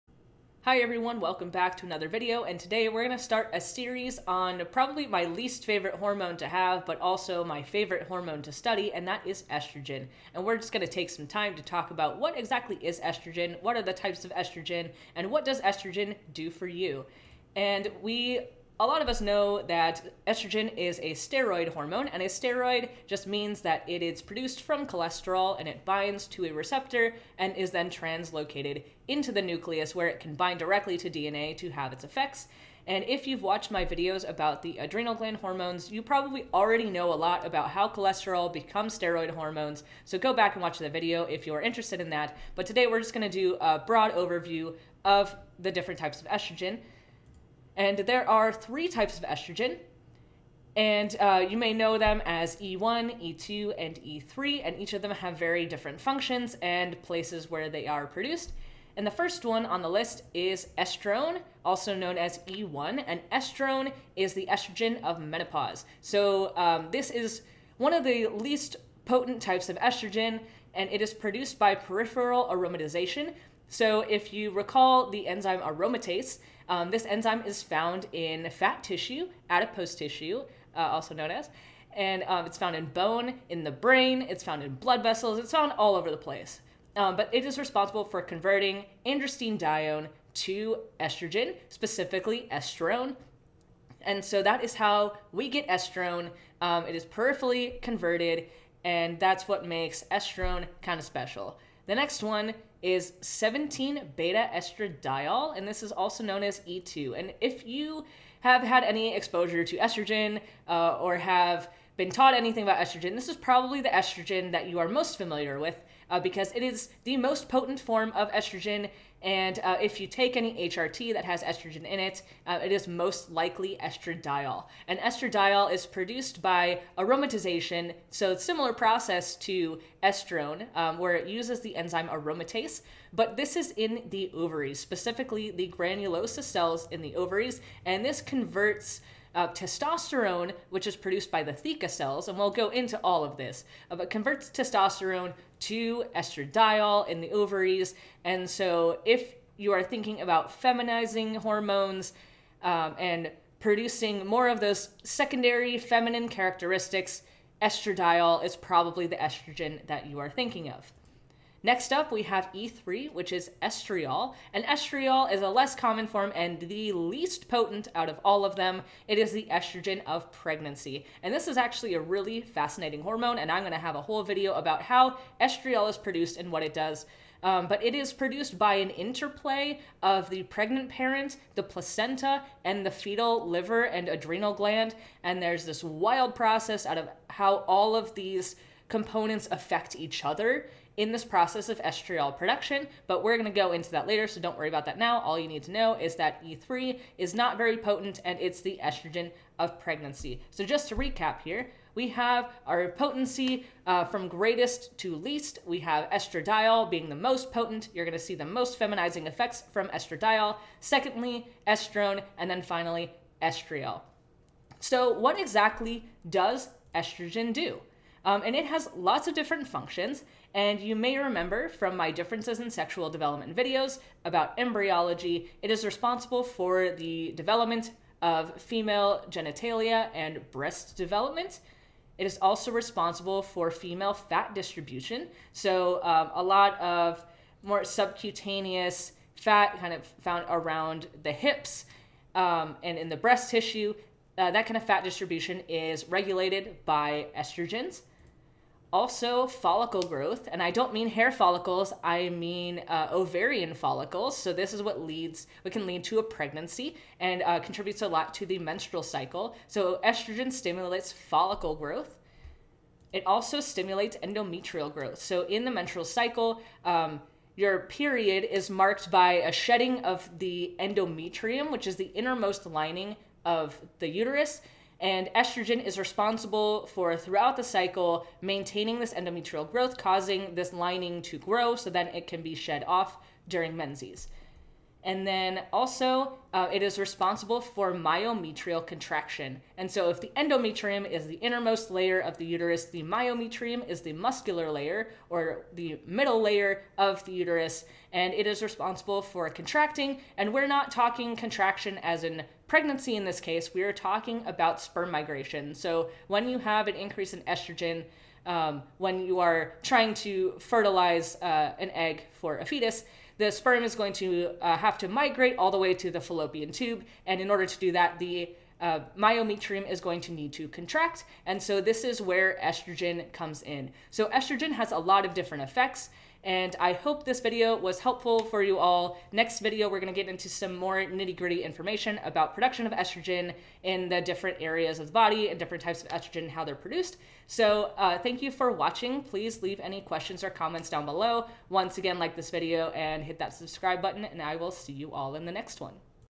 What Are the Different Types of Estrogen?
What is estrogen? How many types are there? This video provides an overview of endogenous estrogens (E1, E2, and E3) including where they are produced, relative potency, and their unique functions.